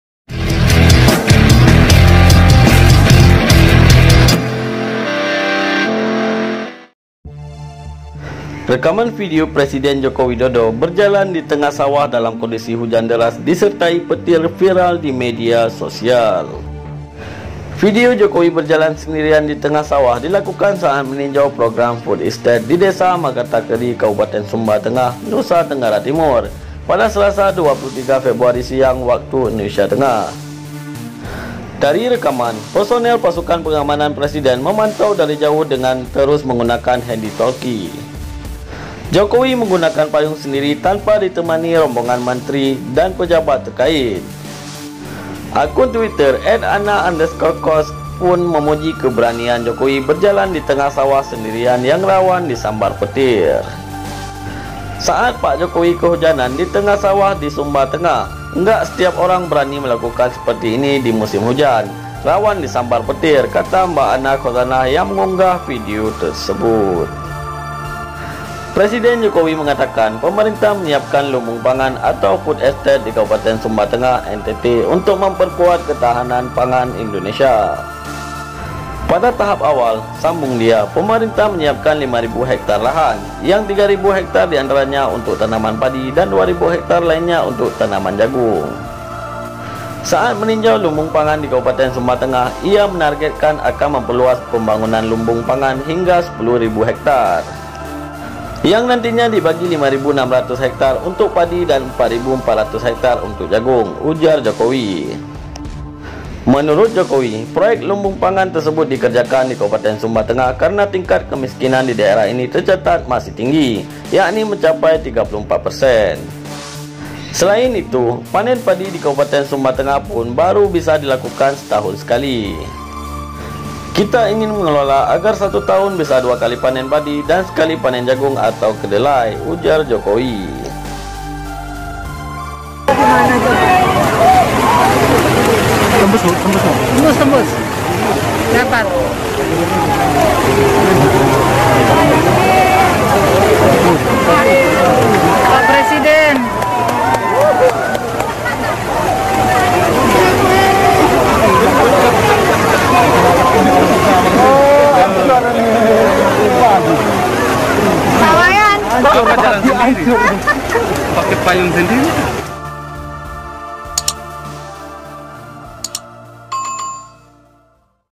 Viral Jokowi Berani Jalan Sendiri di Sawah Saat Hujan Petir
Video Jokowi berjalan sendirian di tengah sawah dilakukan saat meninjau program food estate di Desa Makata Keri, Kabupaten Sumba Tengah, Nusa Tenggara Timur (NTT), Selasa (23/2) siang WITA.

Dari rekaman, personel Pasukan Pengamanan Presiden (Paspampres) memantau dari jauh dengan terus menggunakan handy talky. Jokowi menggunakan payung sendiri tanpa ditemani rombongan menteri dan pejabat terkait. Dilihat Republika hingga Rabu (24/2) pukul 09.00 WIB, video tersebut sudah dilihat sekitar 62 ribu kali.

Akun Twitter @ana_khoz pun memuji keberanian Jokowi berjalan di tengah sawah sendirian yang rawan disambar petir. "Saat Pak @jokowi kehujanan di tengah sawah di Sumba Tengah. Nggak setiap orang berani melakukan seperti ini di musim hujan. Rawan disambar petir," kata Mbak Ana Khozanah yang menggunggah video tersebut.

Presiden Jokowi mengatakan, pemerintah menyiapkan lumbung pangan atau food estate di Kabupaten Sumba Tengah, NTT untuk memperkuat ketahanan pangan Indonesia. Pada tahap awal, sambung dia, pemerintah menyiapkan 5.000 hektare lahan, yang 3.000 hektare di antaranya untuk tanaman padi dan 2.000 hektare lainnya untuk tanaman jagung.

Saat meninjau lumbung pangan di Kabupaten Sumba Tengah, ia menargetkan akan memperluas pembangunan lumbung pangan hingga 10 ribu hektare. “Yang nantinya dibagi 5.600 hektare untuk padi dan 4.400 hektare untuk jagung,” ujar Jokowi saat memberikan keterangan pers usai meninjau food estate di Kabupaten Sumba Tengah, NTT, Selasa (23/2).

Menurut Jokowi, proyek lumbung pangan tersebut dikerjakan di Kabupaten Sumba Tengah karena tingkat kemiskinan di daerah ini tercatat masih tinggi, yakni mencapai 34 persen. Selain itu, panen padi di Kabupaten Sumba Tengah pun baru bisa dilakukan setahun sekali. "Kita ingin mengelola agar satu tahun bisa dua kali panen padi dan sekali panen jagung atau kedelai," ujar Jokowi.

Selain ke Sumba Tengah, Jokowi juga mengunjungi Kabupaten Sikka, NTT. Di sini, Jokowi meresmikan Bendungan Napun Gete di Desa Ilinmedo, Kecamatan Waiblama, Sikka, Selasa sore.